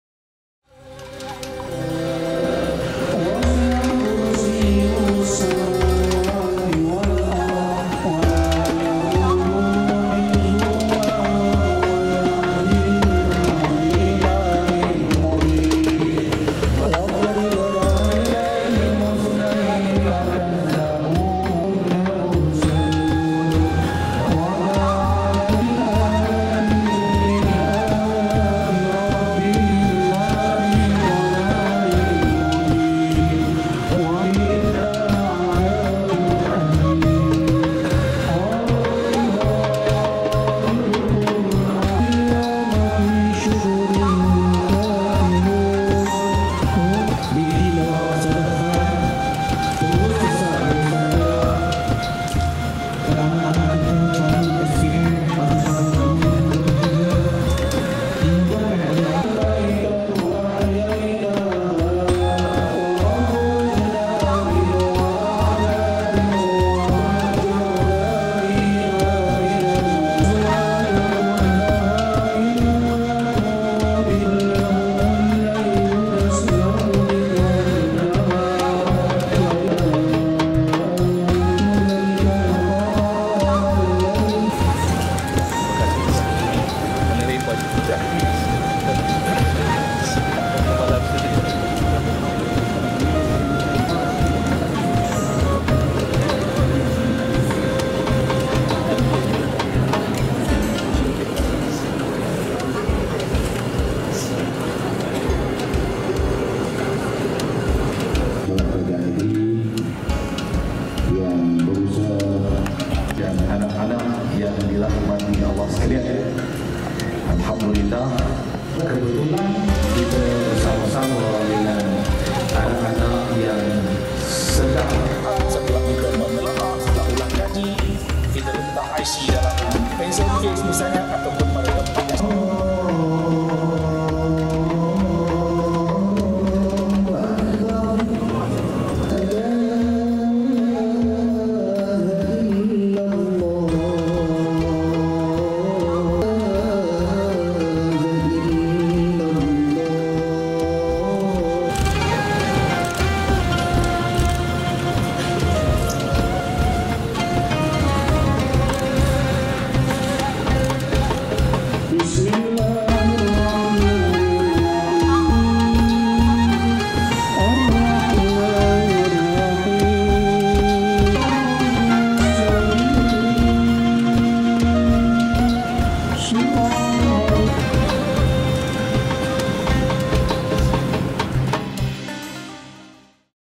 MGPerak: Saarani Sertai Solat Hajat Calon SPM, STPM, STAM
IPOH – Seramai 750 pelajar, ibu bapa serta guru menyertai Program Bacaan Yasin dan Solat Hajat Perdana Sempena Peperiksaan Sijil Pelajaran Malaysia (SPM), Sijil Tinggi Pelajaran Malaysia (STPM) dan Sijil Tinggi Agama Malaysia (STAM) 2023/2024 di Laman Seri Ridzuan, malam tadi.

Menteri Besar Perak, Datuk Seri Saarani Mohamad bersama isteri, Datin Seri Aezer Zubin turut hadir menyantuni program tersebut. - MGPerak